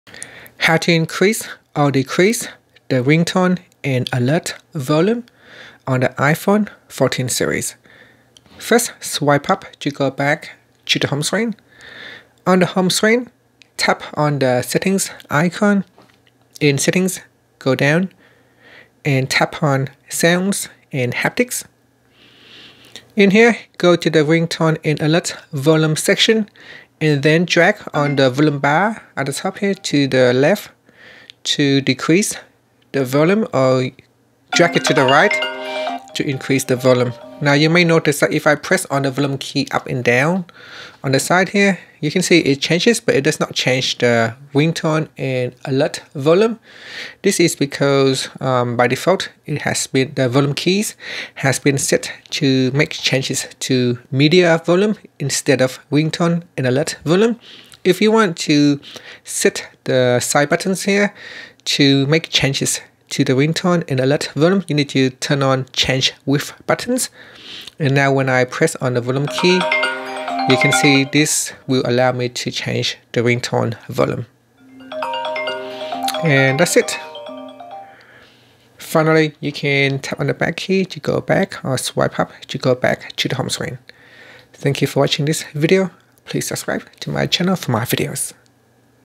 iPhone 14's/14 Pro Max: How to Increase/Decrease Ringtone & Alert Volume
Learn how you can increase or decrease ringtone and alert volume on the iPhone 14/14 Pro/14 Pro Max/Plus.